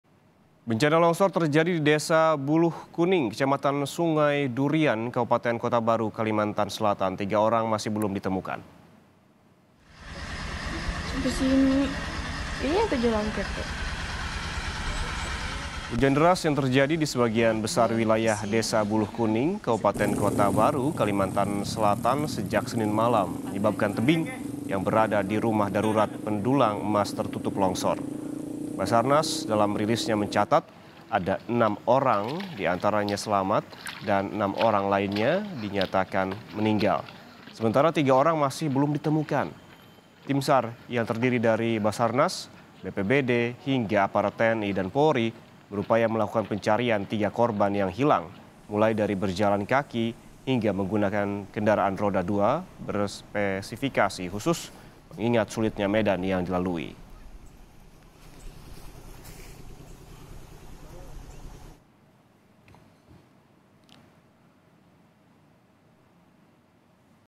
Tanah Longsor Terjang Kotabaru, 6 Orang Tewas, 3 Lainnya Masih Dicari
KALSEL, KOMPAS.TV - Longsor terjadi di Desa Buluh Kuning, Kecamatan Sungai Durian, Kabupaten Kotabaru, Kalimantan Selatan.
Tiga orang masih belum ditemukan. Hujan deras yang terjadi di sebagian besar wilayah Desa Buluh Kuning, Kabupaten Kota Baru, Kalimantan Selatan sejak Senin malam menyebabkan tebing yang berada di rumah darurat pendulang emas tertutup longsor.
Basarnas dalam rilisnya mencatat ada 6 orang diantaranya selamat, dan 6 orang lainnya dinyatakan meninggal.
Sementara tiga orang masih belum ditemukan. Tim SAR yang terdiri dari Basarnas, BPBD hingga aparat TNI dan Polri berupaya melakukan pencarian 3 korban yang hilang mulai dari berjalan kaki hingga menggunakan kendaraan roda dua berspesifikasi khusus mengingat sulitnya medan yang dilalui.